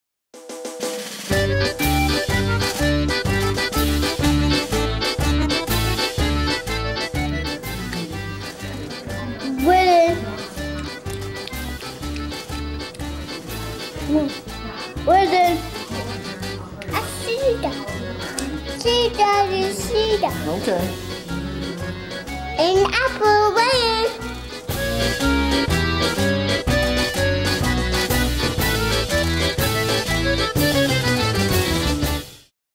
Sadie wins
Sadie does her best Charlie Sheen. While eating an apple.